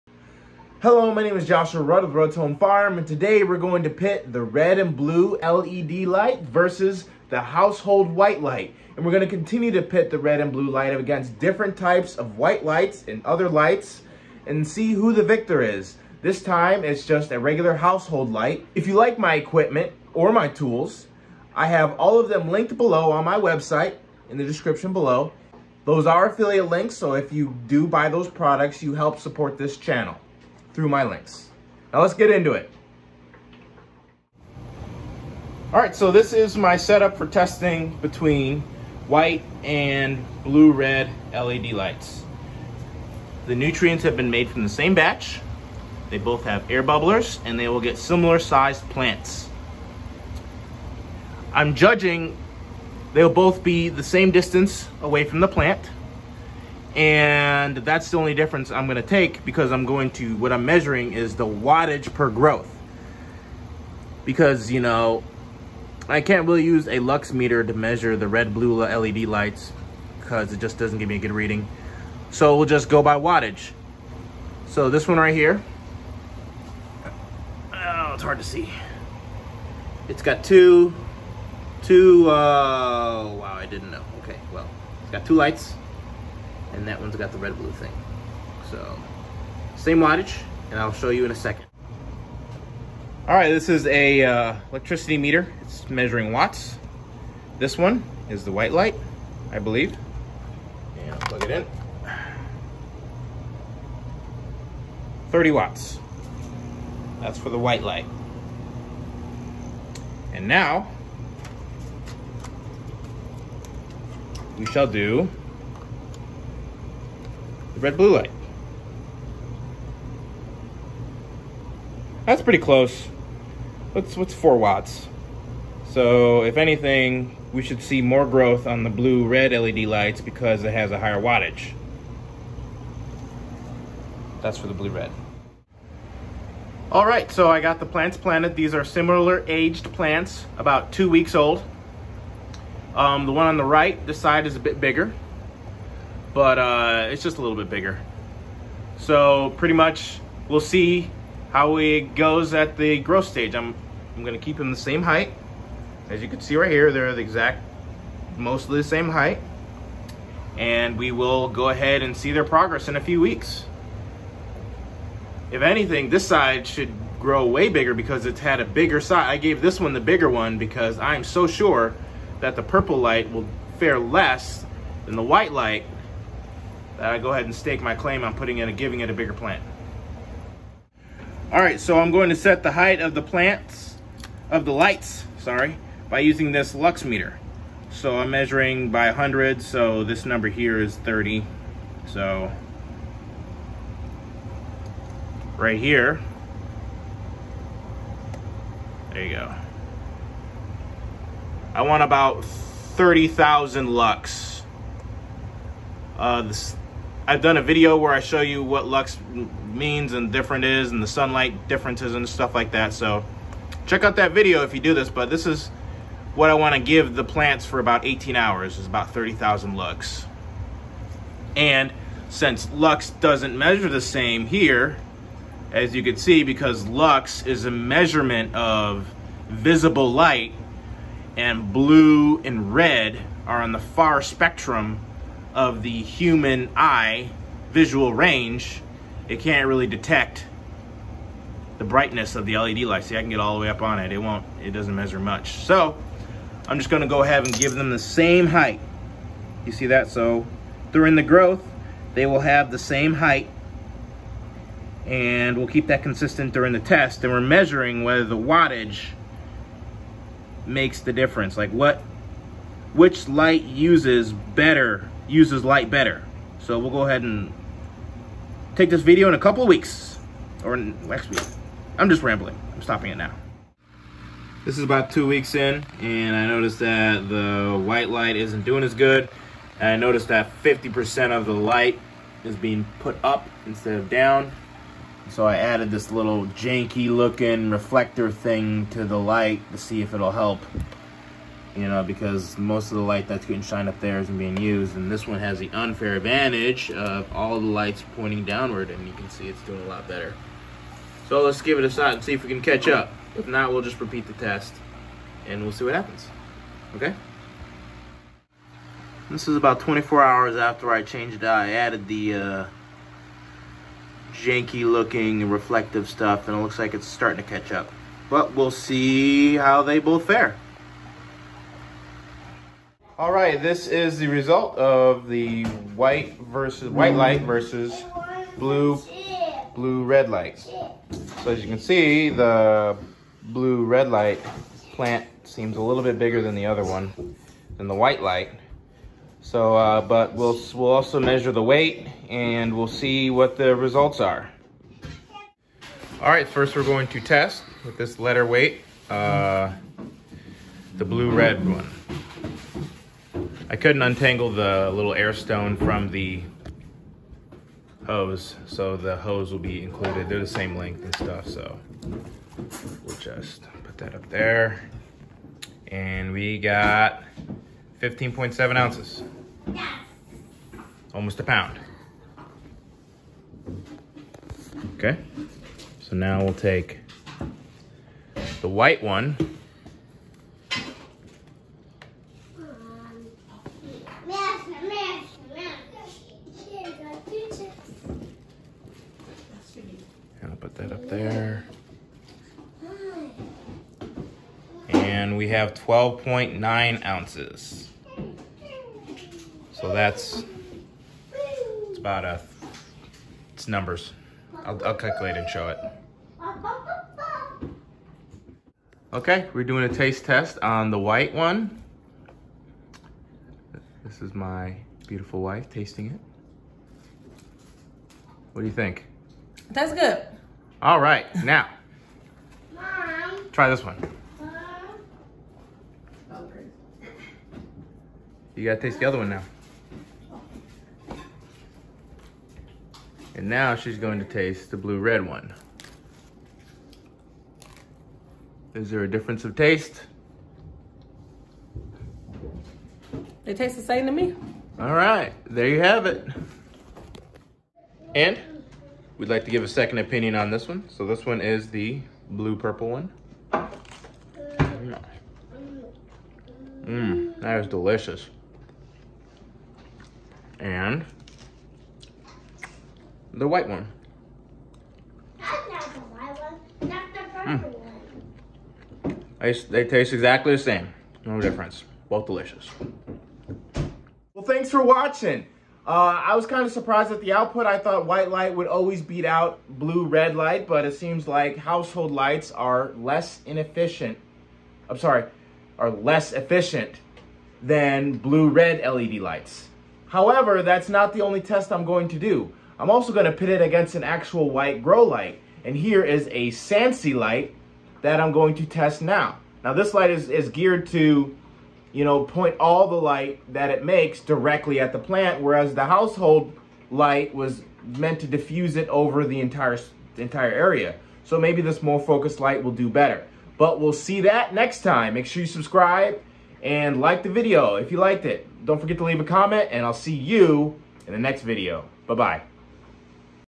Grow Light Test: White vs Blue-Red LED light (Surprising result!)
Today I will be showing you the results of my grow test between a household white light and a red-blue grow light. I will test a Sansi light vs blue-red in the next test.